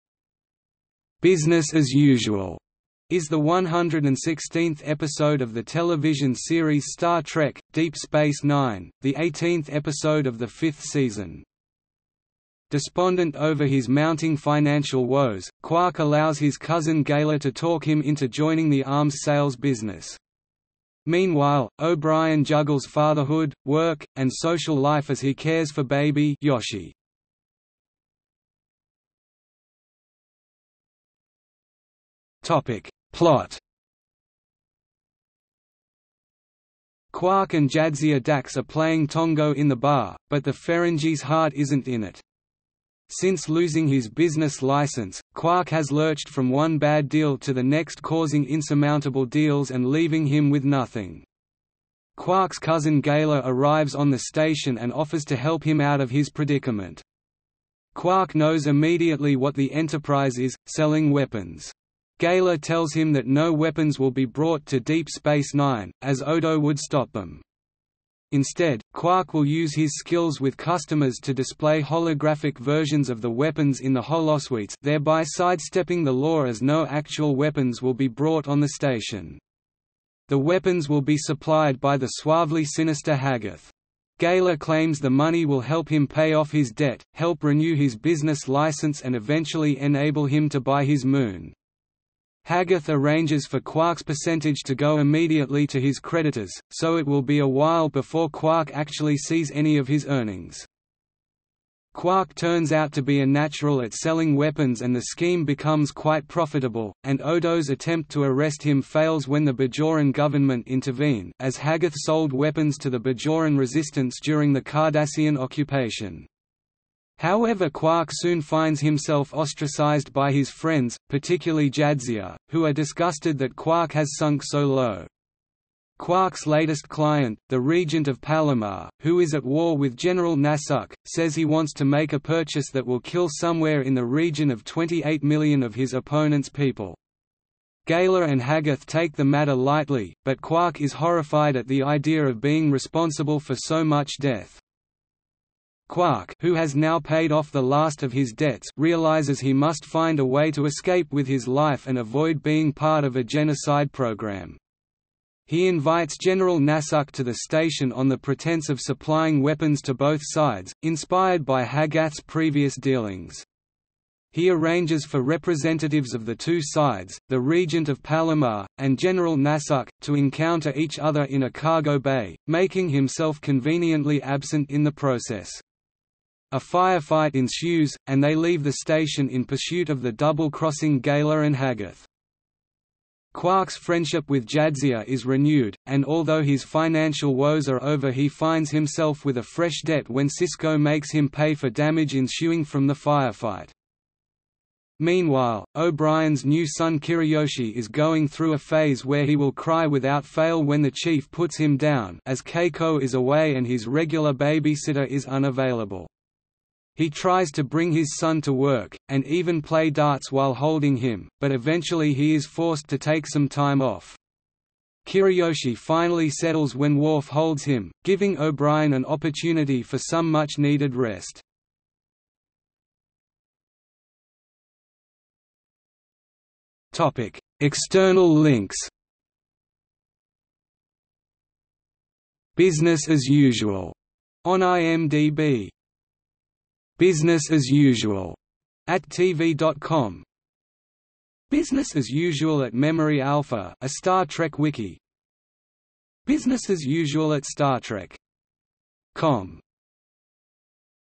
Business as Usual (Star Trek: Deep Space Nine) | Wikipedia audio article
00:00:28 Plot
00:03:43 External links

- Socrates

SUMMARY
"Business as Usual" is the 116th episode of the television series Star Trek: Deep Space Nine, the 18th episode of the fifth season.
Despondent over his mounting financial woes, Quark allows his cousin Gaila to talk him into joining the arms sales business. Meanwhile, O'Brien juggles fatherhood, work, and social life as he cares for baby 'Yoshi.